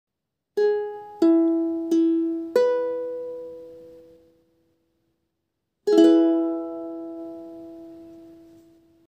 E Major Chord (2-0-4-1) on a High G Ukulele Arpeggio and Strum
A video demonstrating the E major chord on an ukulele with reentrant GCEA tuning. First played as an arpeggio from string 4 to 1 at 90bpm followed by a single down strum. For additional clarity there is a chord diagram in the bottom right corner of the video.